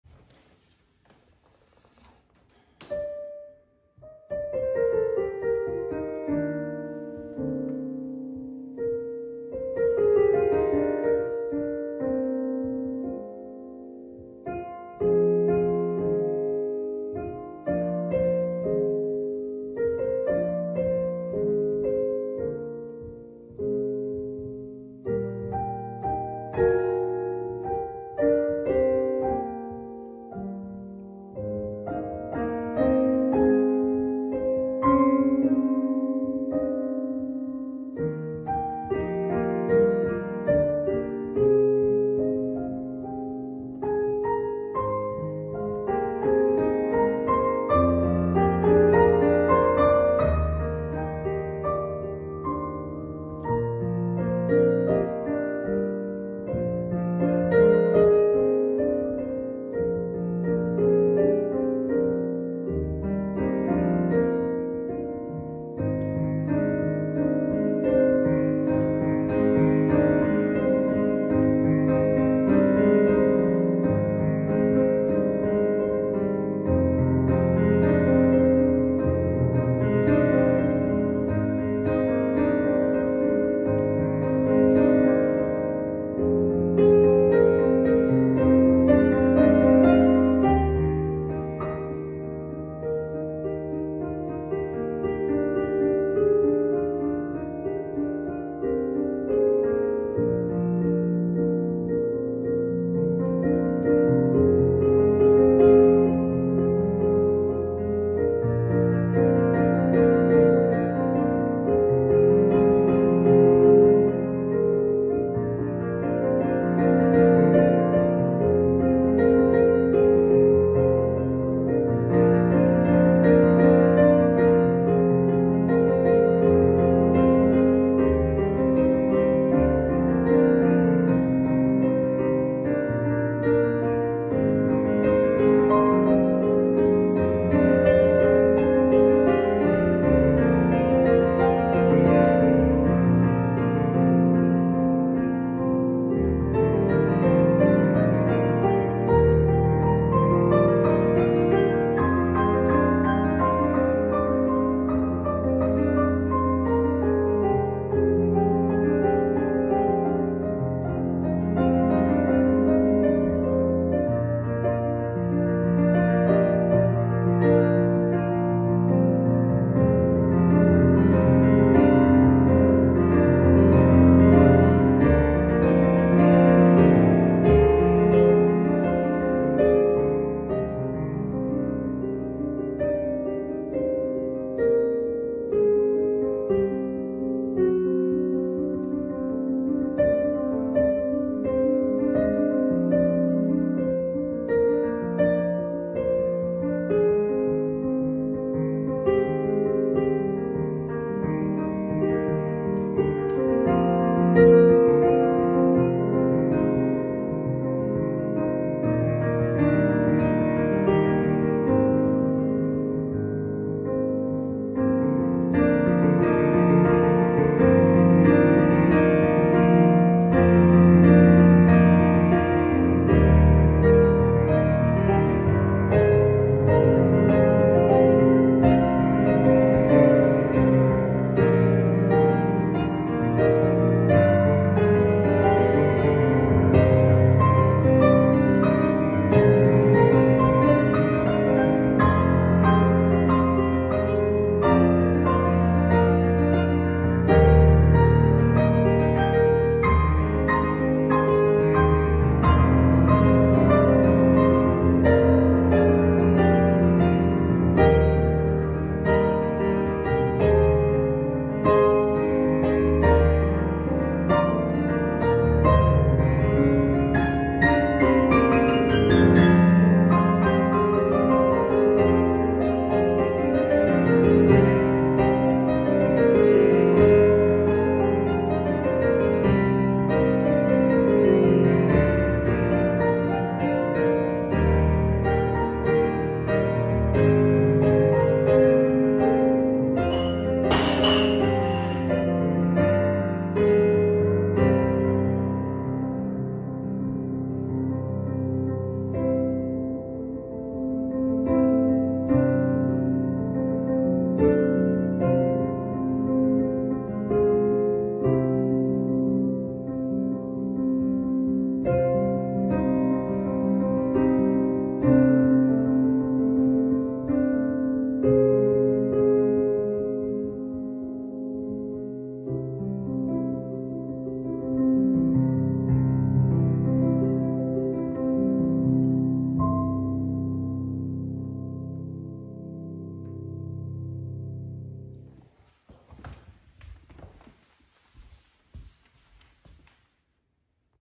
Maine Coon - my cat Theo is my best fan!
mainecoon piano composer funnyvideo While playing on an easy Sunday afternoon, my cat Maine Coon decided to join me and jumped on my piano while I was playing!